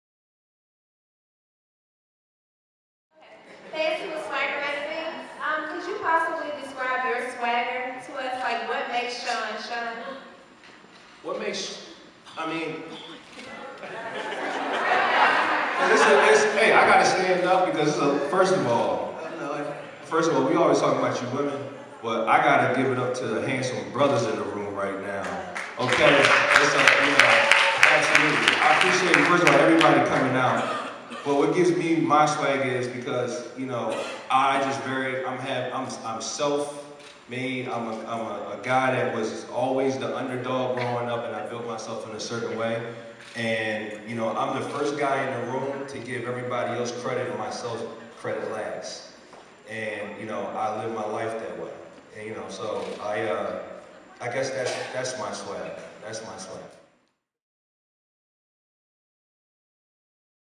MMIH Shawn Bullard Talks His Swagger
Real estate magnate and star of WE TV's  Match Made in Heaven talks his swagger during the private screening in ATL with a Q&A led by Claudia Jordan of The Rickey Smiley Morning Show and Bravo's Real Housewives of Atlanta.